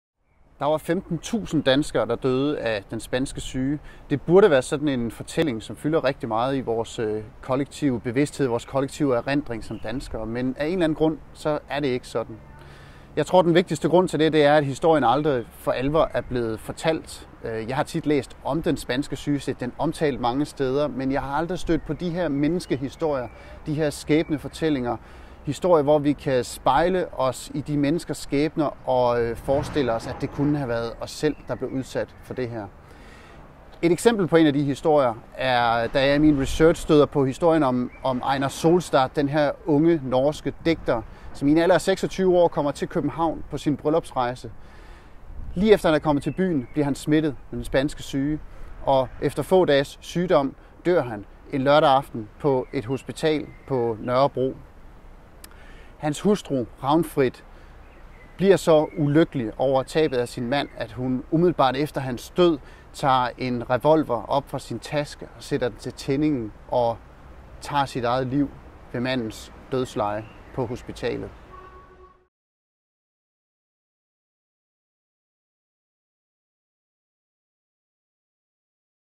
Den spanske syge og den norske tragedie
Gennem en lang række øjenvidneberetninger kommer vi helt tæt på den angst, sorg og uhygge, der lå tungt over Danmark under epidemien. Vi hører om tragiske dødsfald og hårdt ramte familier, men også om de frygtløse sygeplejersker, læger og frivillige hjælpere, der kom patienter til undsætning i hele landet.

Historien trækker tråde helt op til nutiden. Først for nylig fik man rekonstrueret det virus, der var årsag til epidemien. I dag er den spanske syge derfor med til at gøre forskerne og lægerne klogere på, hvad vi skal stille op, når den næste store epidemi rammer os.